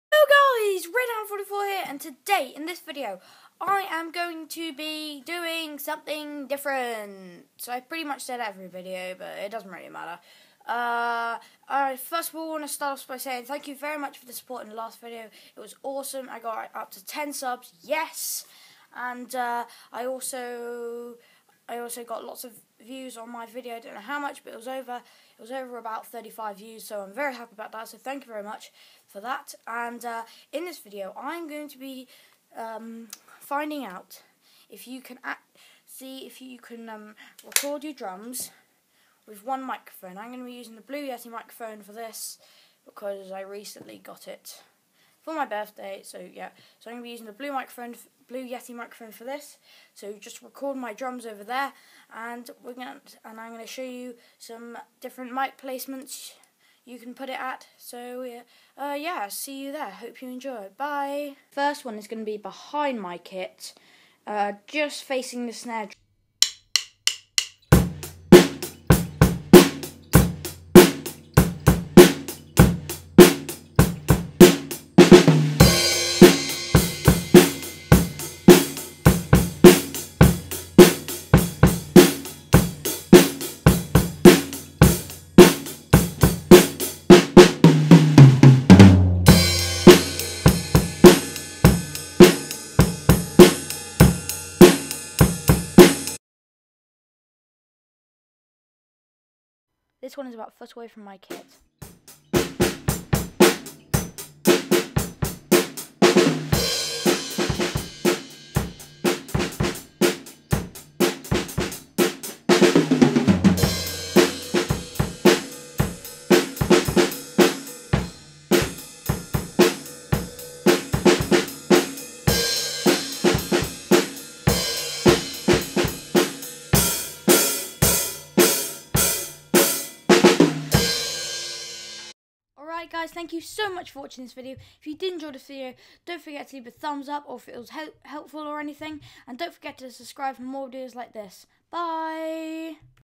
how to record drums with one mic
A sort video on were to put your mic on a drum kit to get a good sound :)